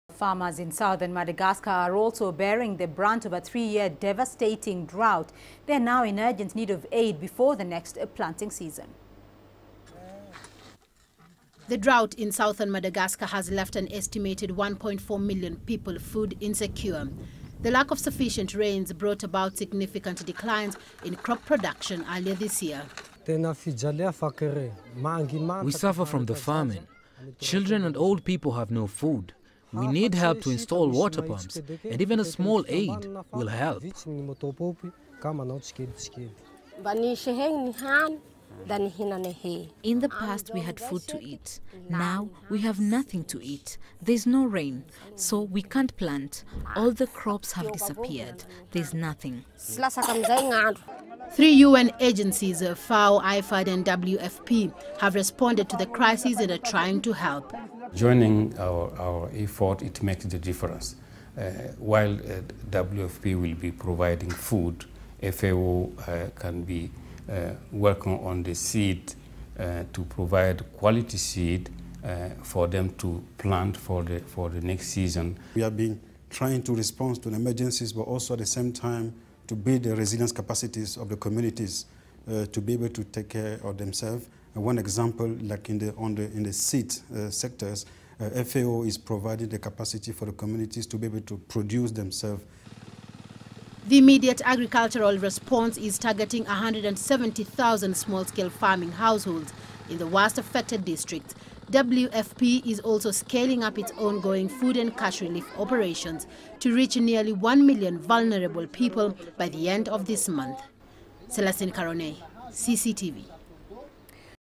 Madagascar local communities urged to plant more drought-resistant crops
Farmers in Southern Madagascar are also bearing the brunt of a three-year devastating drought. They are now in urgent need of aid, before the next planting season.